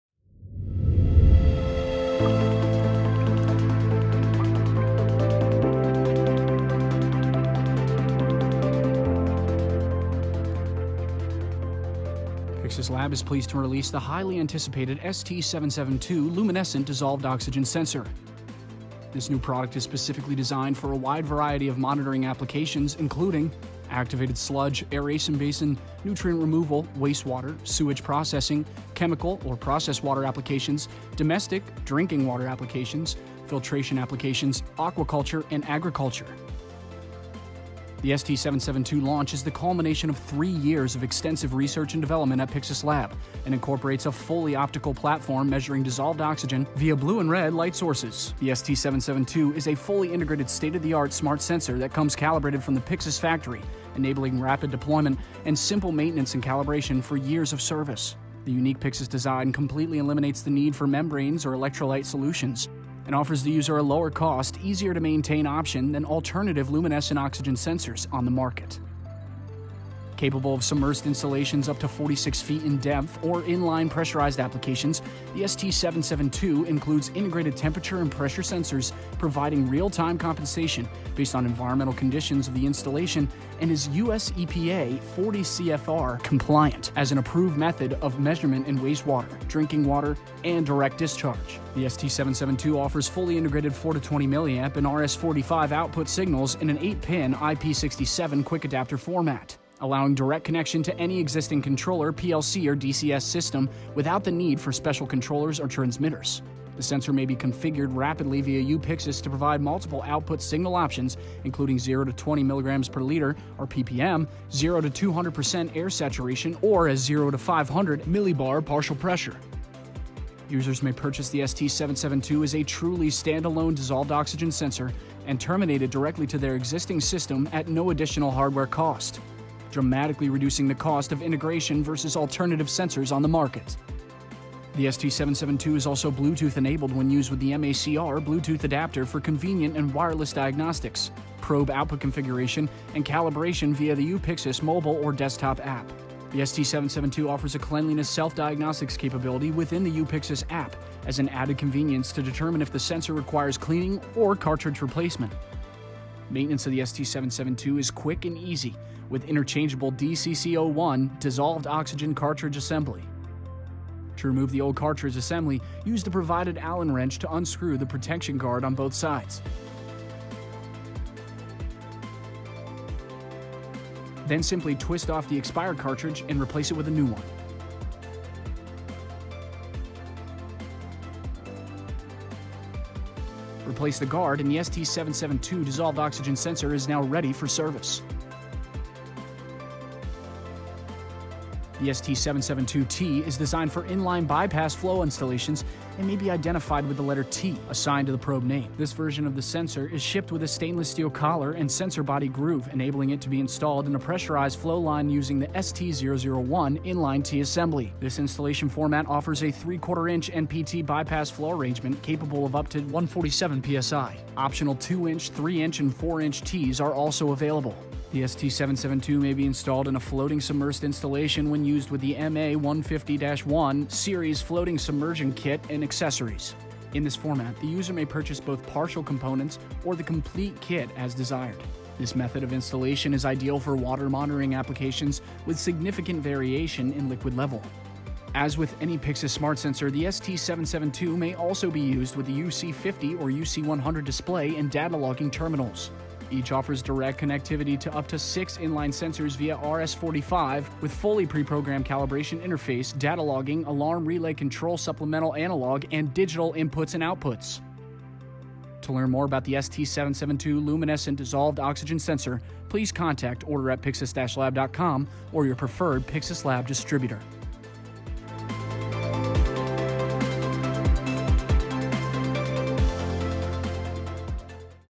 Introducing the ST-772/T Luminescent Dissolved Oxygen Sensor | Pyxis Lab®
After three years of extensive research and development Pyxis Lab® is pleased to formally launch the ST-772 Series luminescent dissolved oxygen sensor for water and process application monitoring.

This new state of the art sensor is applicable for a wide variety of applications including nutrient removal, wastewater, process water, environmental, aquaculture and more.

With a monitoring range of 0.004 – 20.0 mg/L (as dissolved oxygen), this new sensor offers many markets a lower cost, stand alone sensor platform that can be easily integrated into any existing controller, PLC or Distributed Network without the need for supplemental equipment or controllers.